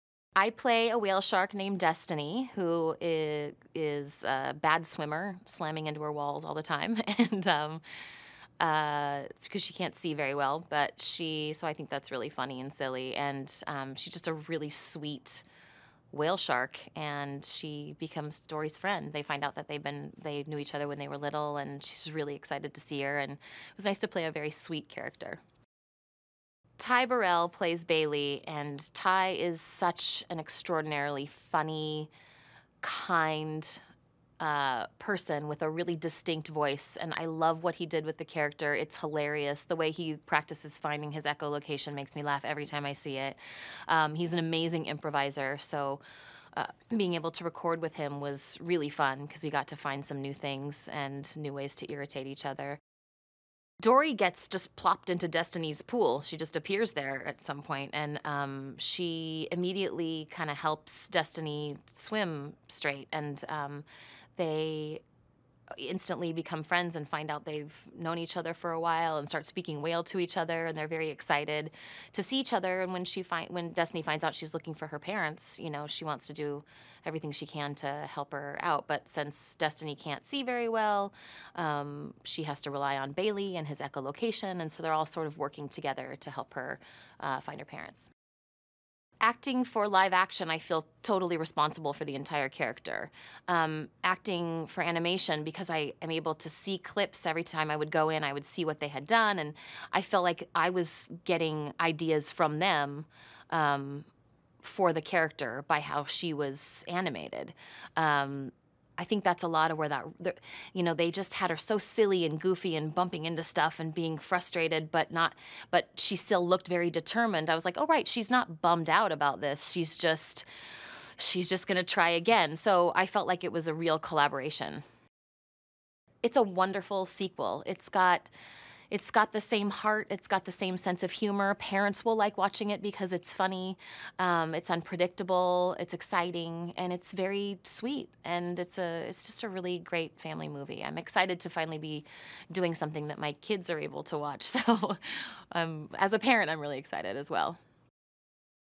Finding Dory: Kaitlin Olson "Destiny" Behind the Scenes Movie Interview | ScreenSlam
Finding Dory: Kaitlin Olson "Destiny" Behind the Scenes Movie Interview